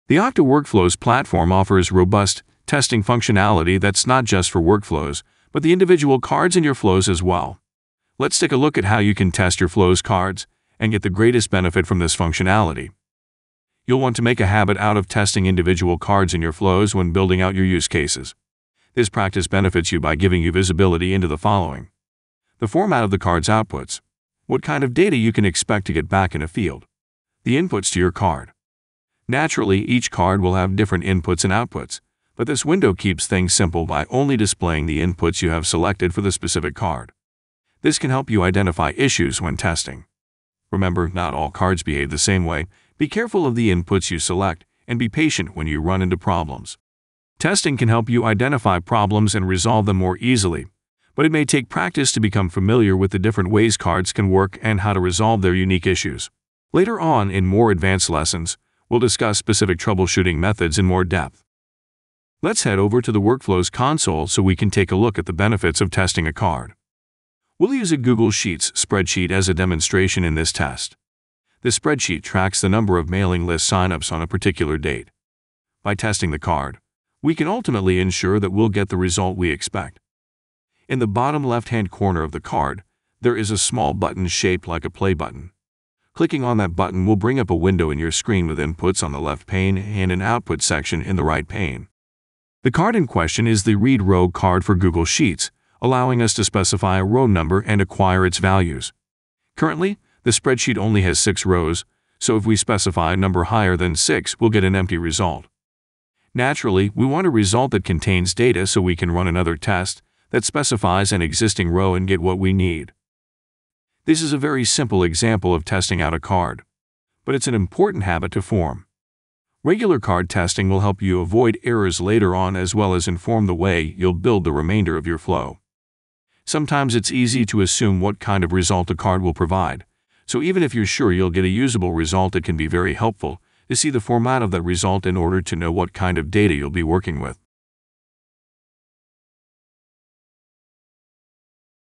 Okta Workflows 109 - Testing Individual Cards
You don't have to wait until you're done building a flow to test your cards. This video walks you through the process of testing individual cards in the Okta Workflows console. Testing cards can inform the way that you may choose to build the remainder of your flow and facilitates real-time adjustments.